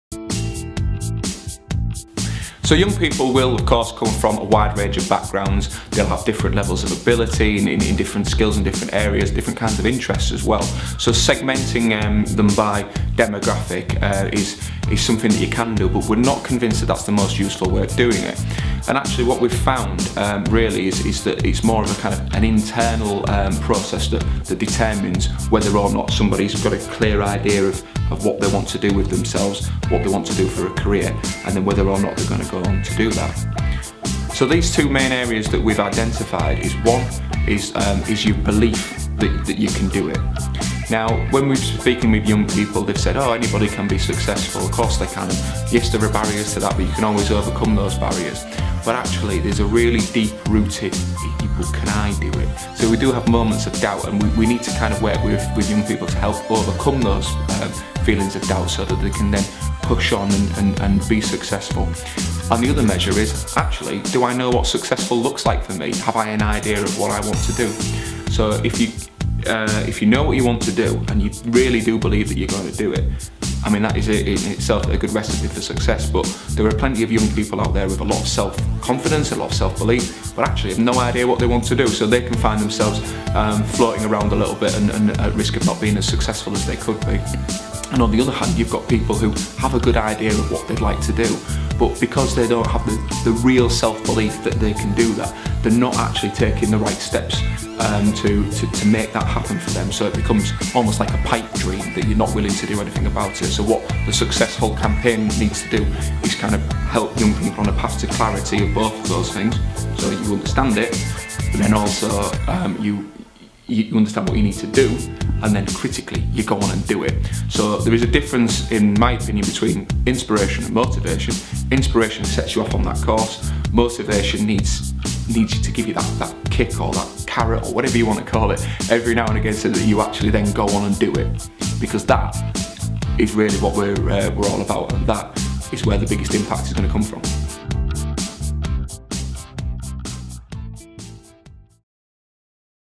Success Soup - Segmenting the target market
A plan seeking investment and support to make a significant positive impact to young people in Hull and build a sustainable model to create jobs and role the programme out nationally.